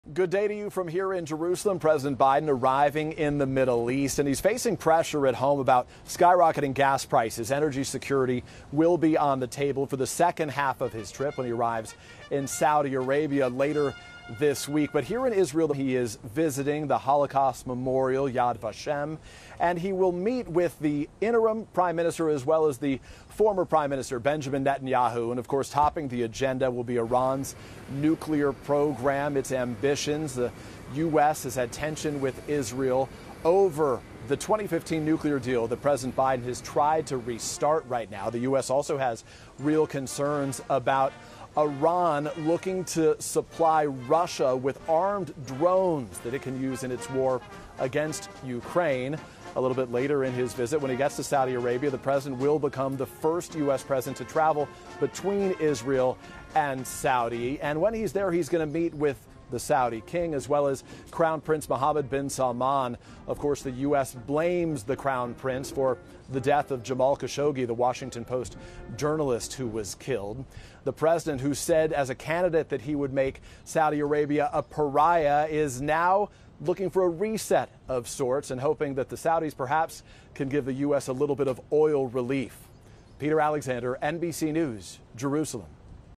President Biden visit to the Middle East
President Biden kicked off his first visit to the Middle East. Expectations are low regarding hopes that the visit might lower tensions between nations. NBC's Peter Alexander reports.

9NEWS (KUSA) is located in Denver, Colorado.